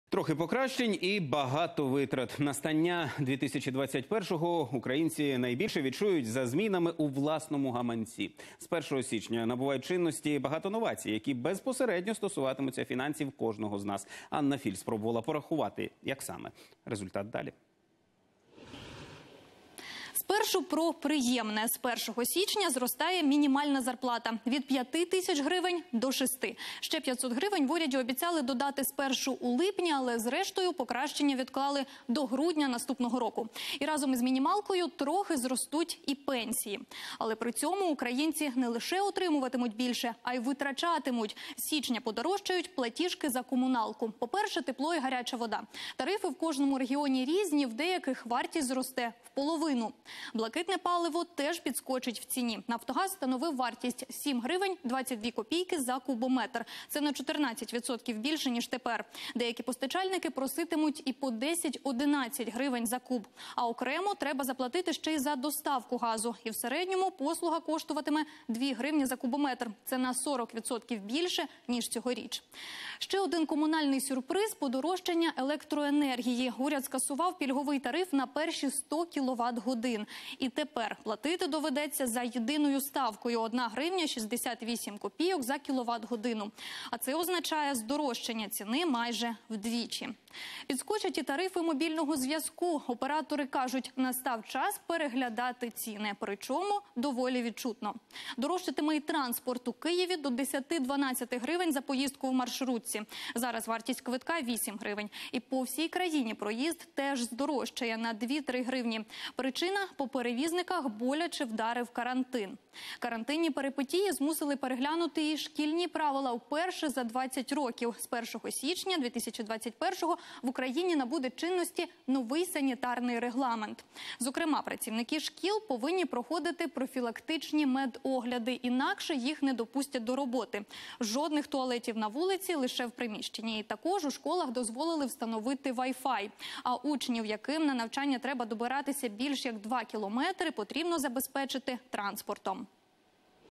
Уряд підготував українцям нові "комунальні сюрпризи"
Настання 2021 року українці найбільше відчують за змінами у власному гаманці - з 1 січня набувають чинності багато новацій, які безпосередньо стосуватимуться фінансів кожного з нас